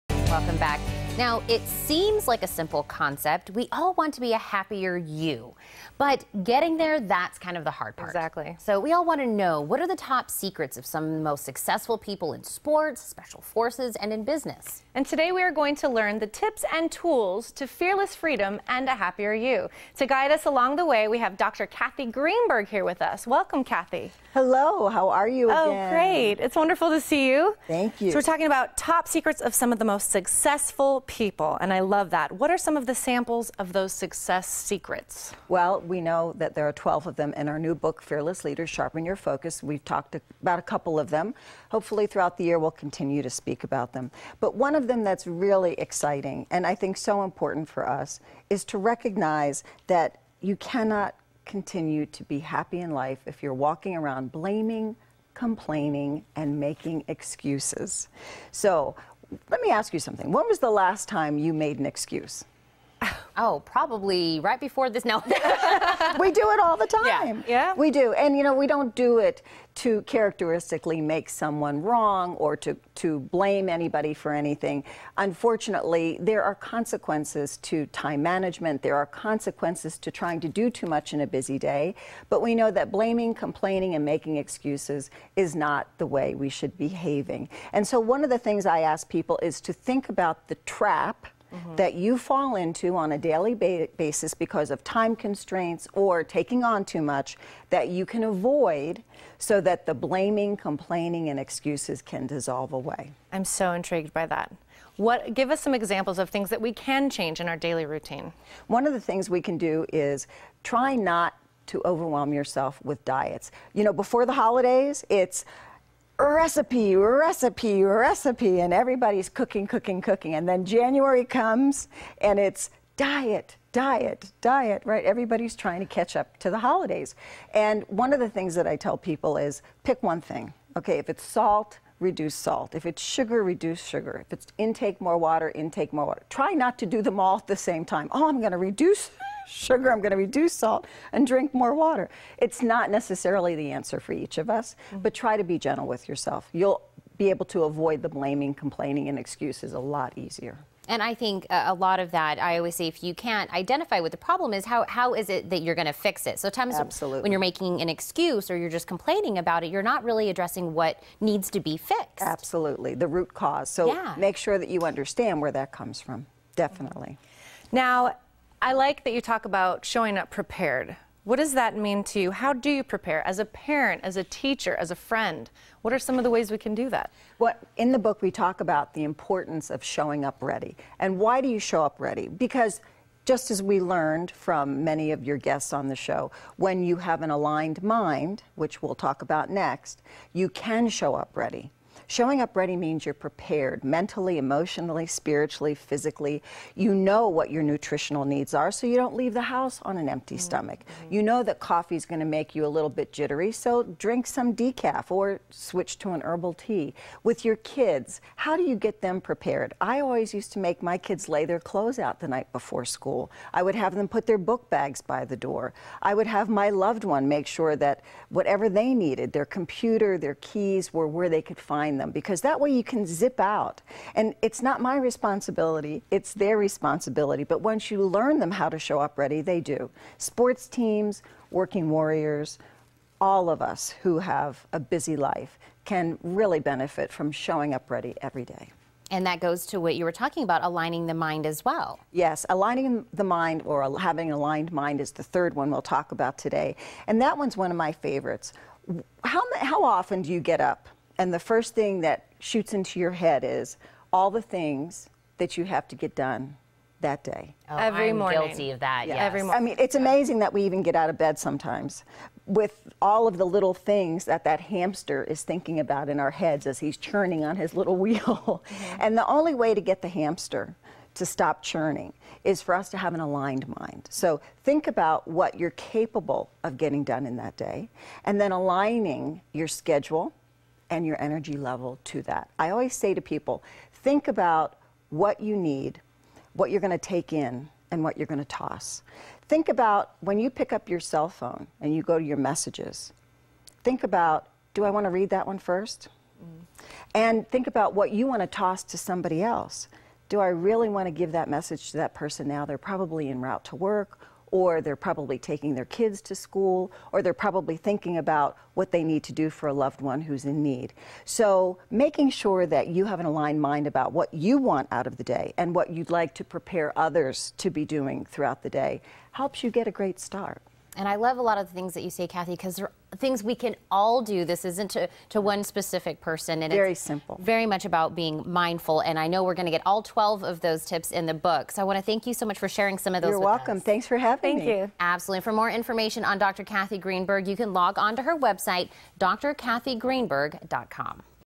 Cathy Greenberg - Fearless Freedom
Top athletes, special operators and the best performers know that being your best means having the courage to take responsibility for your actions.

Cathy Greenberg, the first lady of happiness, shares what can you do in your daily routine to avoid these traps or being a victim to time constraints, circumstances and the needs of those around you.